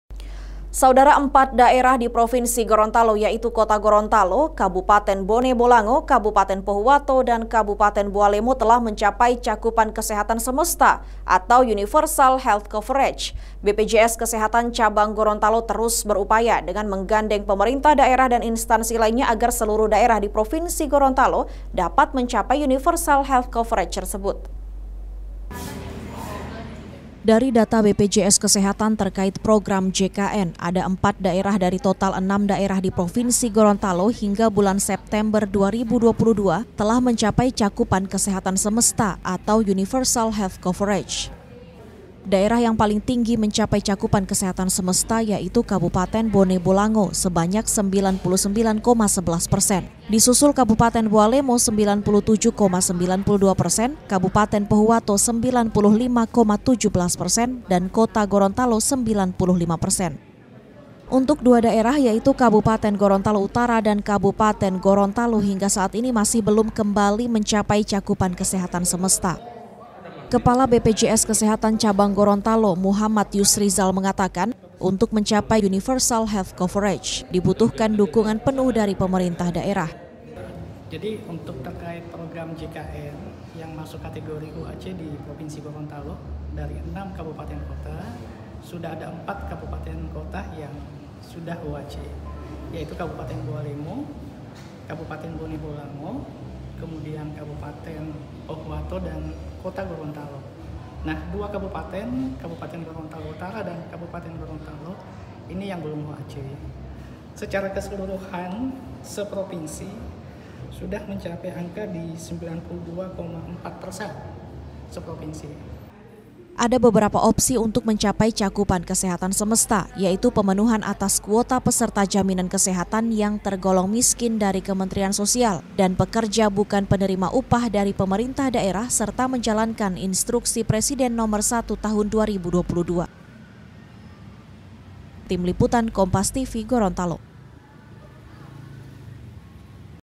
4 Daerah Di Gorontalo Capai Cakupan Kesehatan Semesta
GORONTALO, KOMPAS TV - Empat daerah di Provinsi Gorontalo Yaitu Kota Gorontalo, Kabupaten Bone Bolango, Kabupaten Pohuwato Dan Kabupaten Boalemo telah mencapai cakupan Kesehatan Semesta Atau Universal Health Coverage (UHC).

BPJS Kesehatan Cabang Gorontalo terus berupaya dengan menggandeng pemerintah daerah dan instansi lainnya agar seluruh daerah di Provinsi Gorontalo dapat mencapai Universal Health Coverage tersebut.